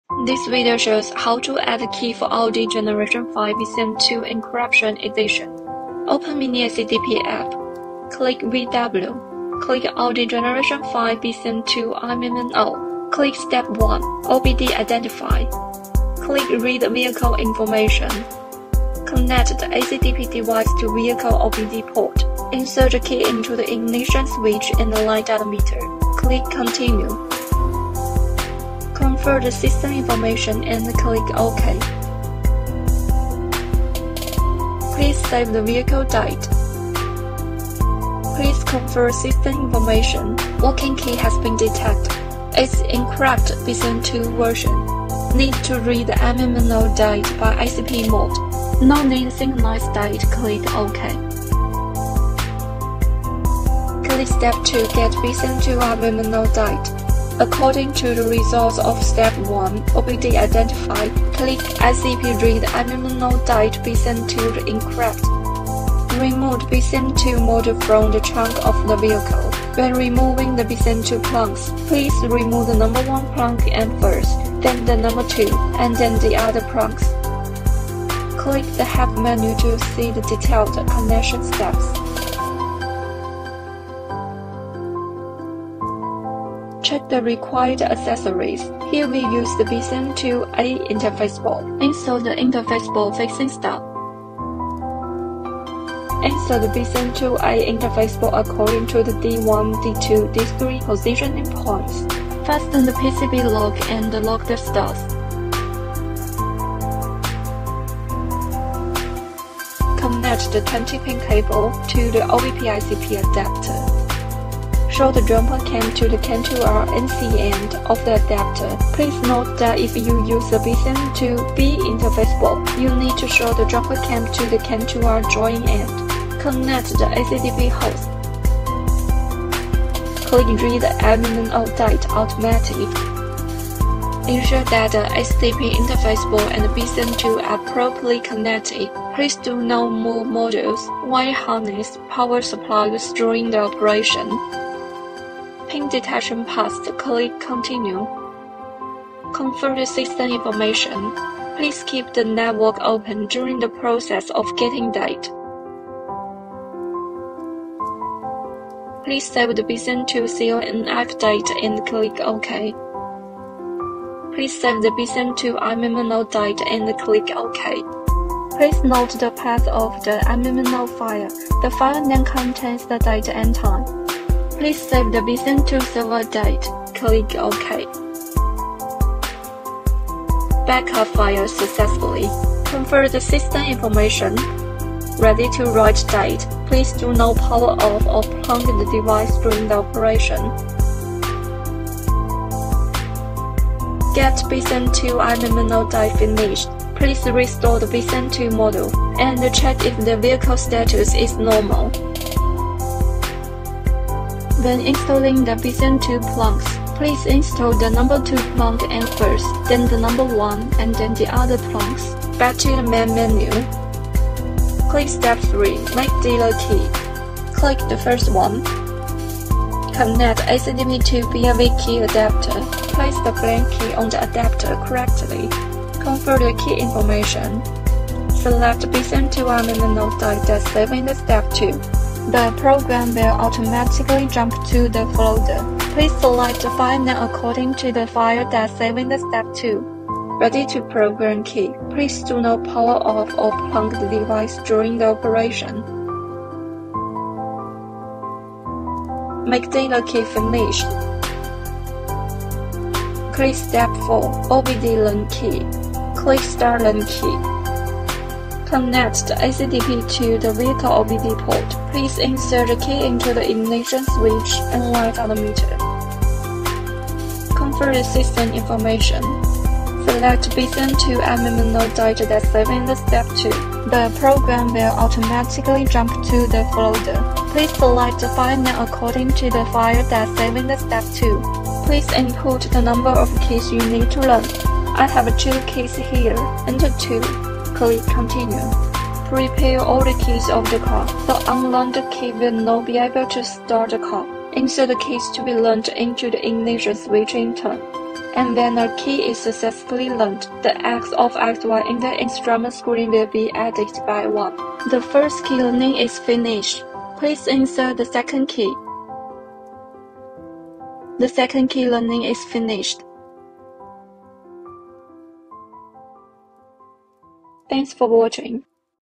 Audi Gen5 BCM2 add key encrypted with ACDP and Module 29 - obd2shop.co.uk
This video shows you how to use Yanhua Mini ACDP and Module 29 to add a key for Audi Gen5 encrypted version BCM2.
Skype: OBD2Tool.co.uk